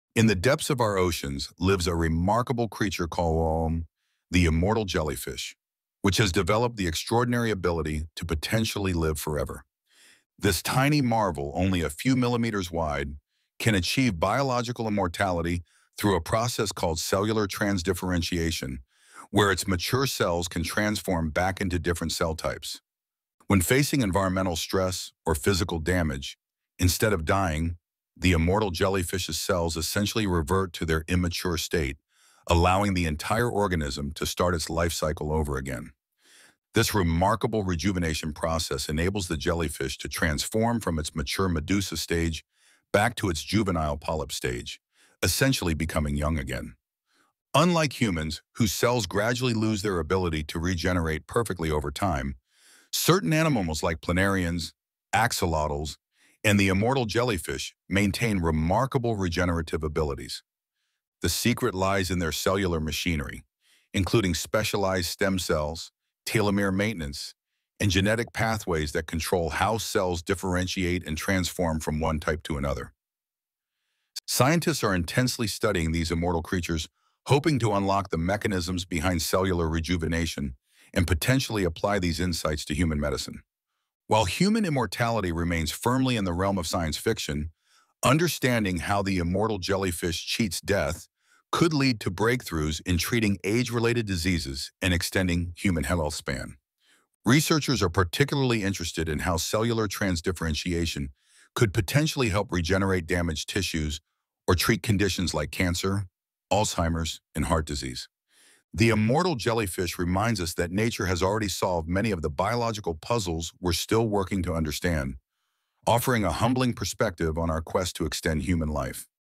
Unlocking the Secrets of the Immortal Jellyfish 🌊✨
Dive deep into the ocean and discover the incredible world of the immortal jellyfish! 🐙💙 This fascinating creature holds the key to biological immortality through a unique process called cellular transdifferentiation. Learn how it can rejuvenate itself and potentially inspire breakthroughs in human medicine. From regenerative capabilities to advanced scientific research, find out how nature might lead us to extend our own healthspan. 🌟🔬